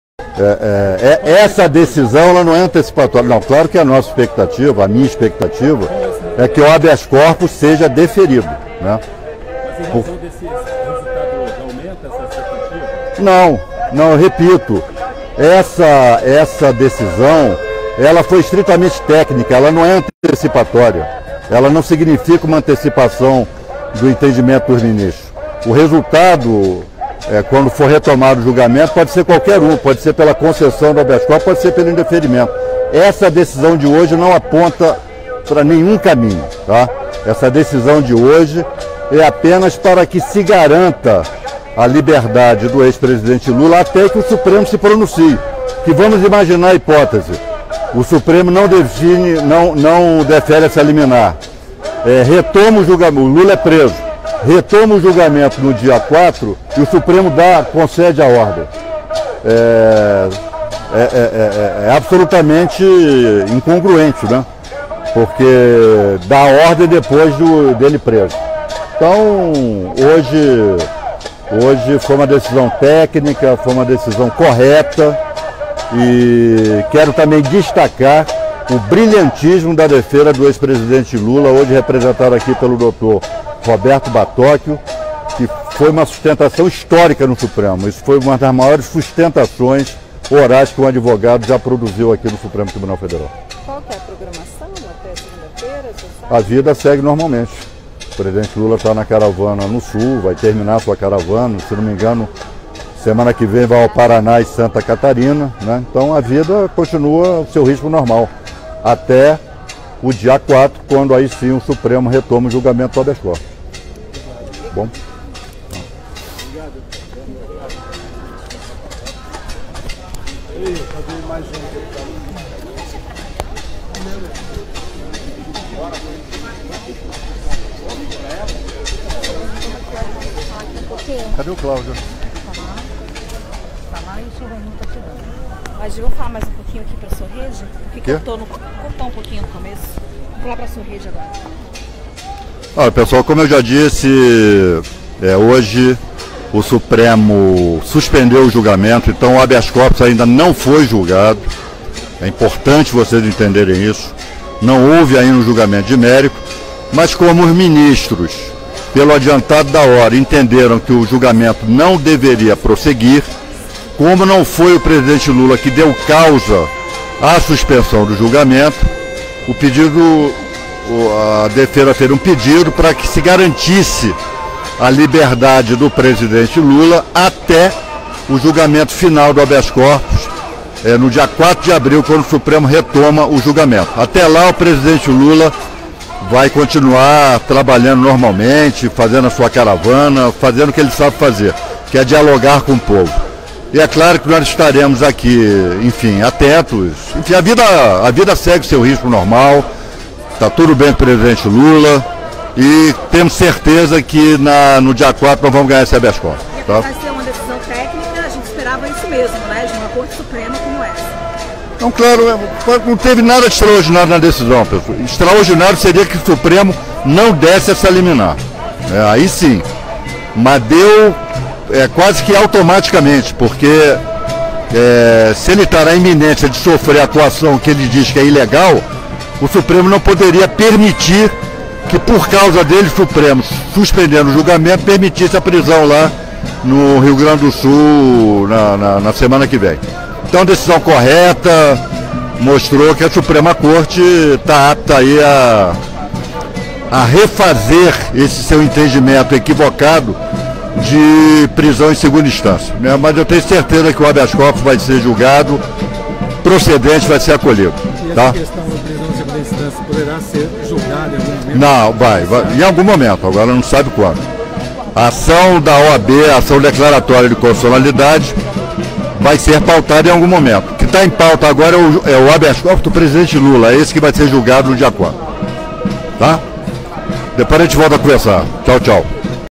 LULALIVRE - O POVO TÁ COM LULA - Parte I- Deputado Wadih Damous (PT-RJ) fala direto do STF
sobre o julgamento do Habeas Corpus de Lula. Maioria dos ministros do STF aceitou a liminar apresentada pela defesa de Lula que suspende a prisão do ex-presidente até o julgamento do mérito do habeas corpus da defesa; julgamento do mérito acontece no dia 4 de abril.

Publicado Face: 22/03/2018